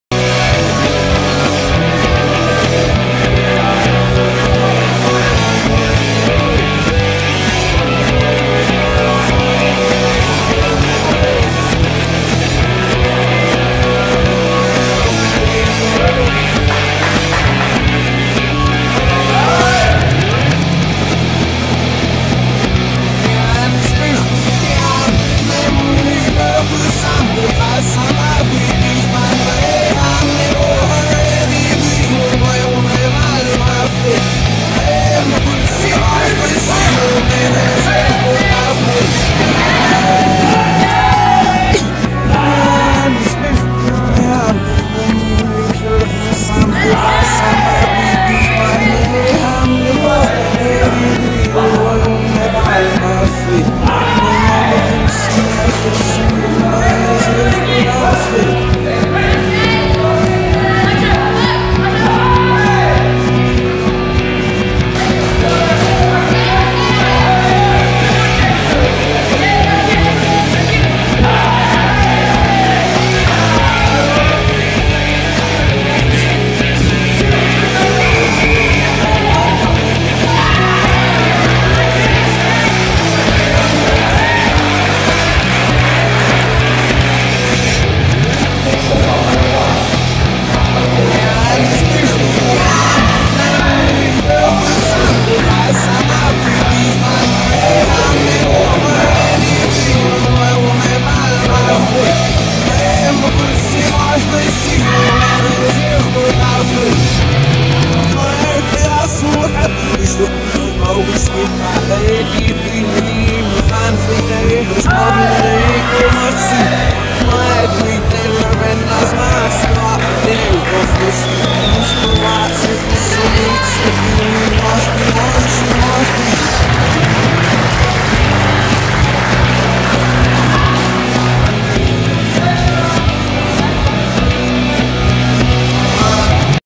gkr wgtn rd1 2017 03 26 r2 instructor male kata 05d bassai dai
GKR Karate
Wellington, New Zealand
Round 1
Porirua 5024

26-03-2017